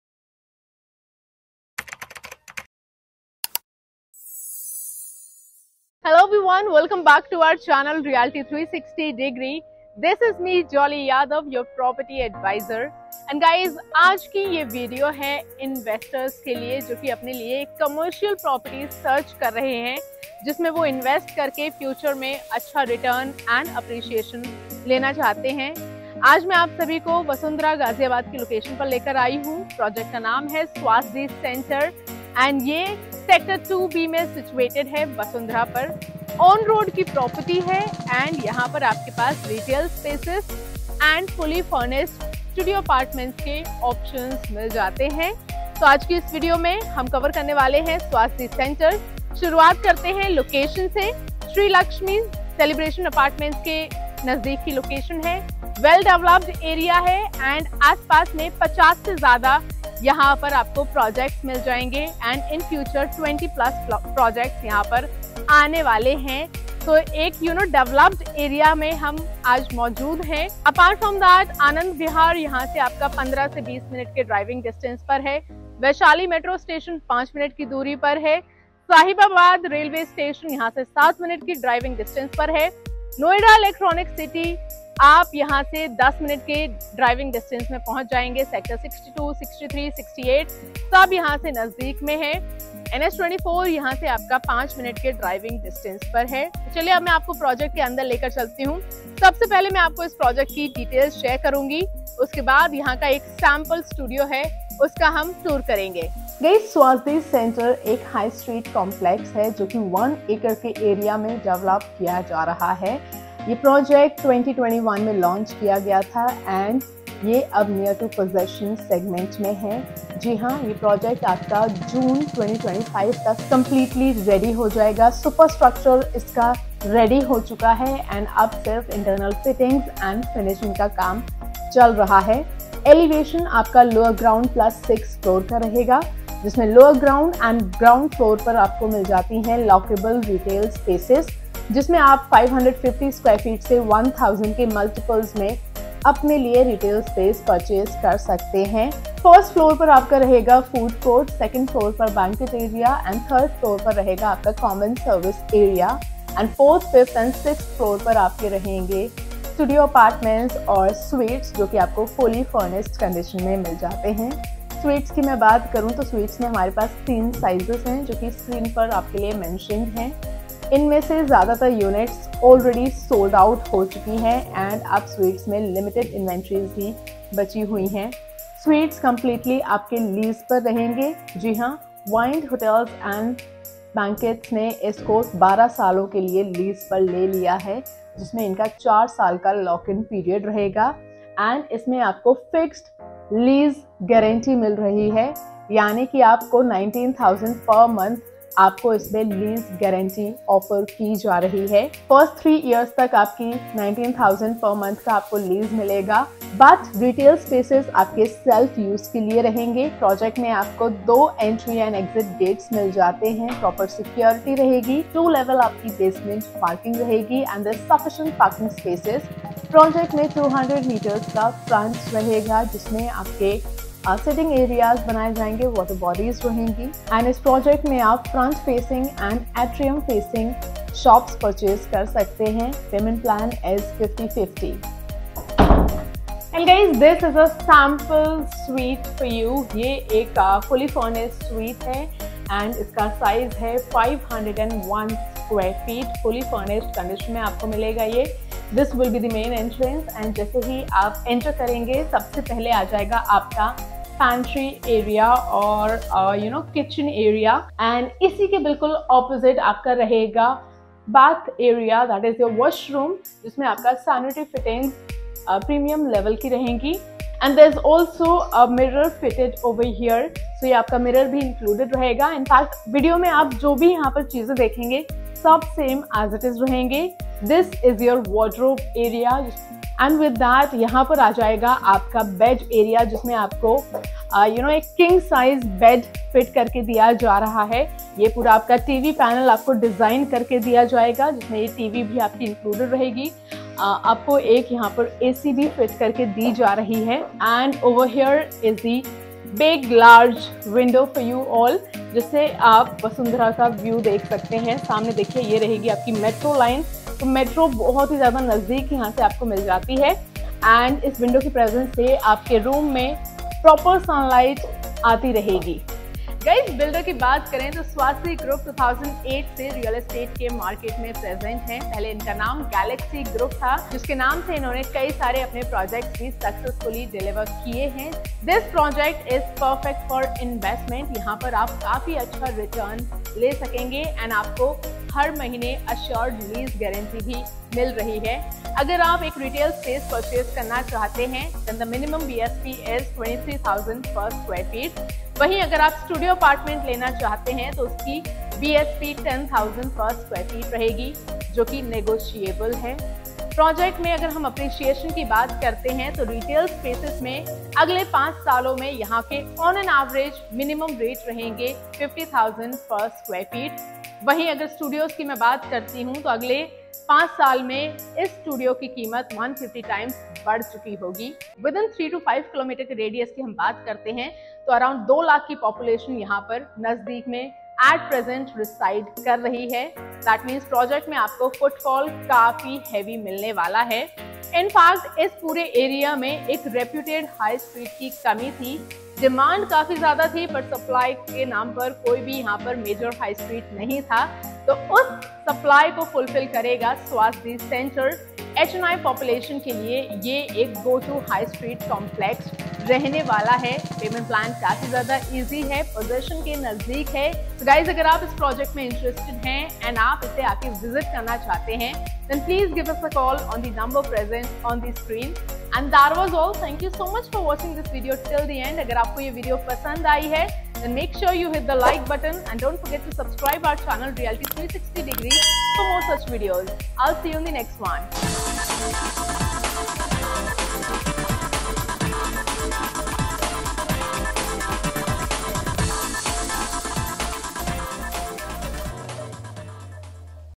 Highstreet Retail SAWASDEE CENTRE Sec 2B Vasundhara | Fully Loaded Studio Apartments & Retail
Retails has all lockable spaces
Size ranges between 550 sqft to 1000 sqft
Price ranges between 23000 to 26000 psf

Studio
Size 463, 501and 502
Fully furnished with lease for 12 years
Brand  wyndd hotels and banquets
Fixed lease of 19000 per month for 36 months
4 years lock in period
 BSP: 10000 PSF
PP : 40:60
Lease 175 psf